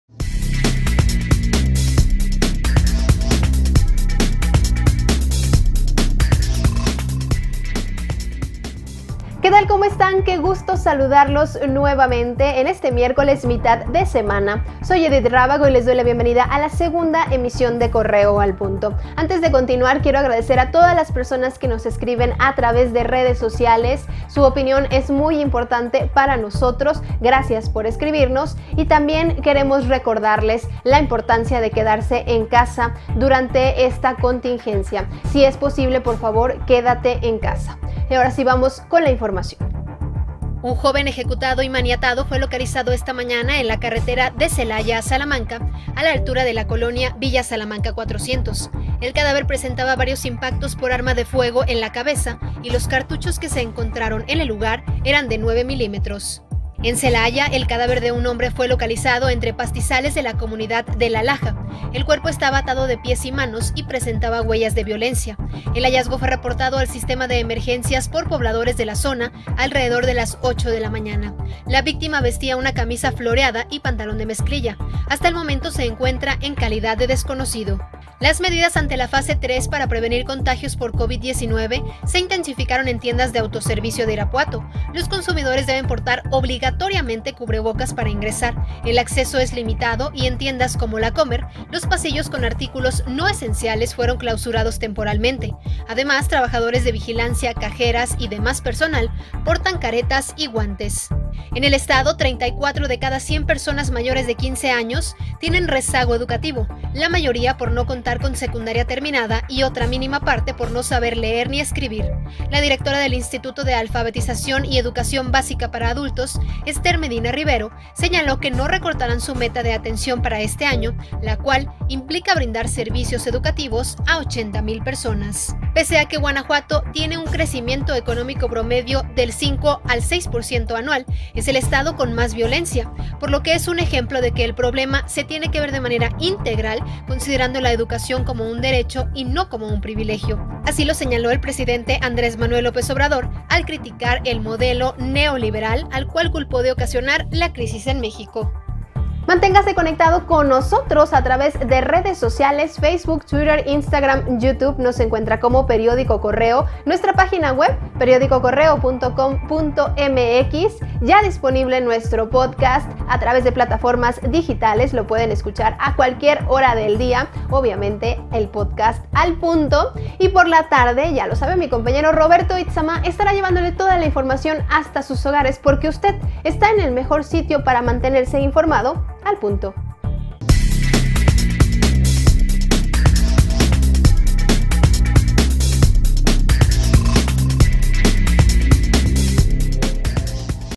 Segunda Emisión
⁃ Dejan cuerpo maniatado y con disparos en la cabeza en Salamanca
⁃ Celaya Encuentran cadáver entre pastizales en comunidad La Laja
 ⁃ Intensifican medidas sanitarias en tiendas de autoservicio
⁃ En Guanajuato 34 de cada 100 personas tienen rezago educativo: INAEBA
⁃ Nacional ¿De qué sirvió el crecimiento en Guanajuato?: AMLO ____________________________________________